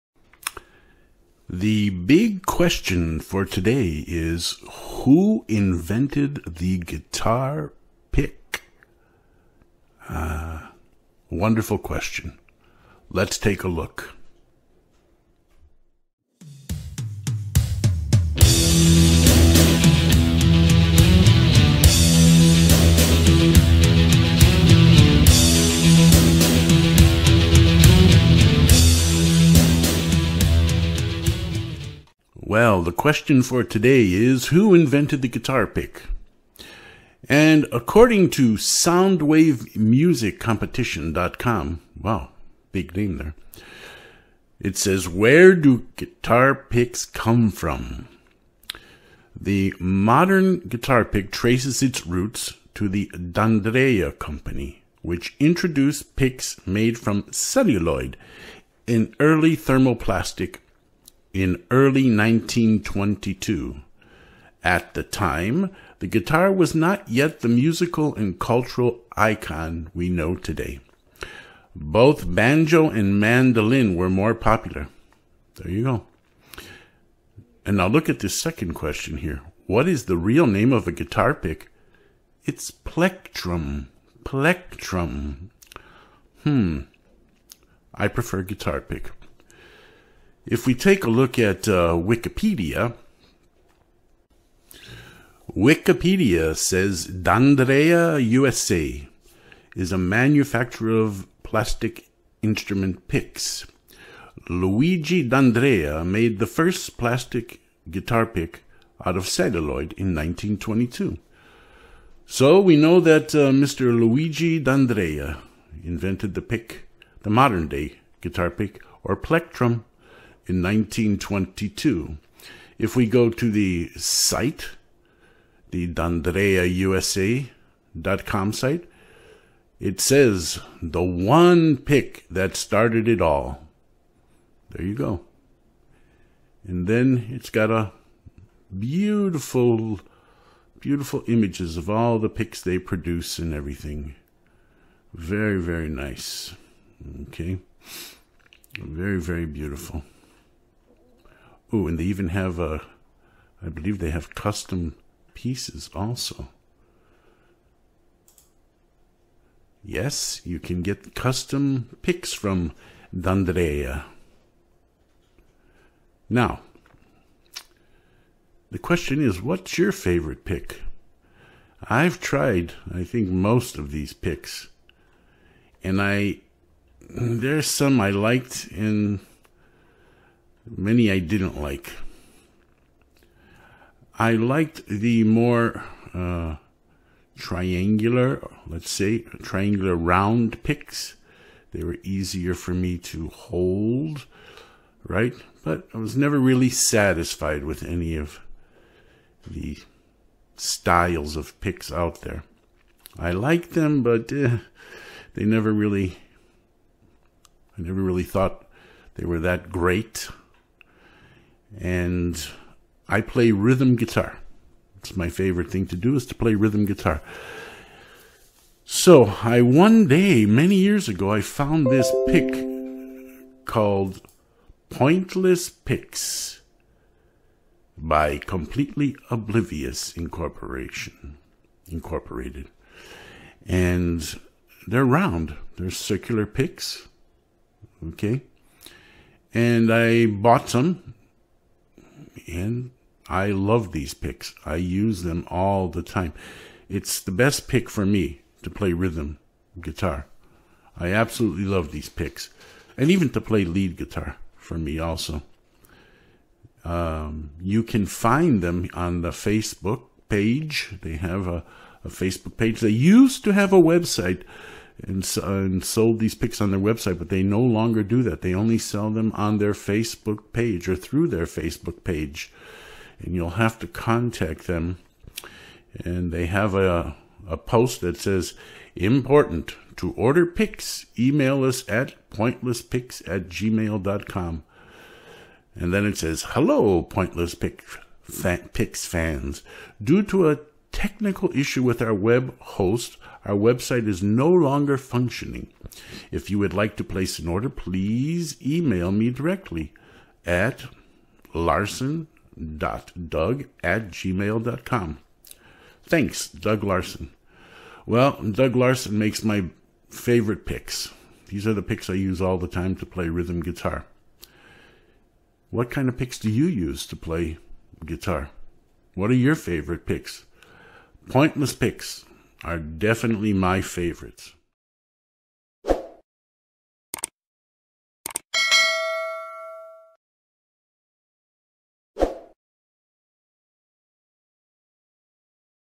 Who Invented The Guitar Pick?
An answer to the question... Who invented the guitar pick? Plus a mention of my favorite guitar pick. It's pointless.